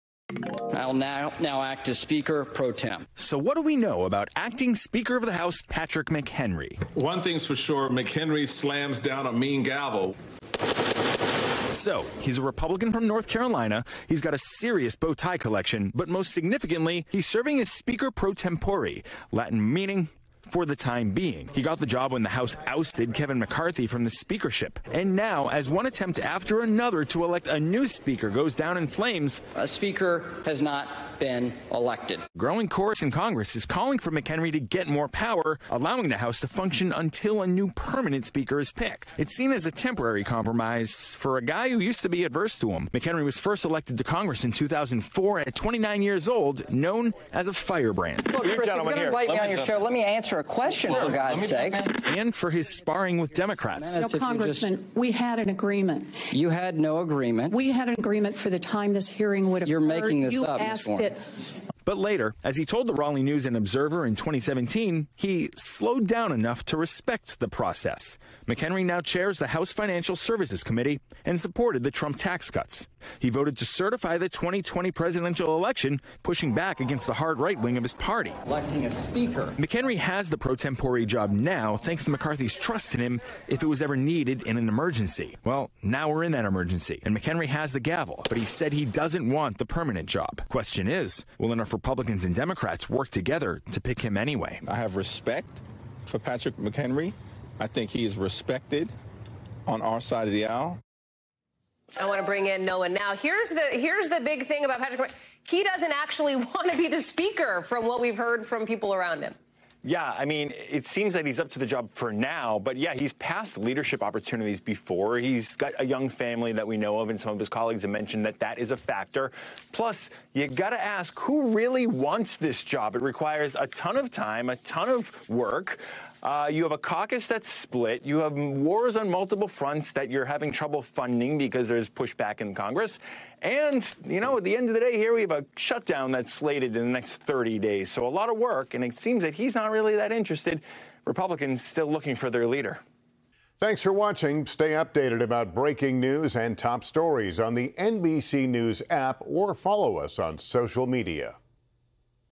Support grows to give more power to acting House Speaker Patrick McHenry
Noah Pransky details everything we know about acting Speaker of the House Patrick McHenry. Bipartisan support is growing for the Republican representative from North Carolina to be the temporary speaker until a new permanent speaker is picked.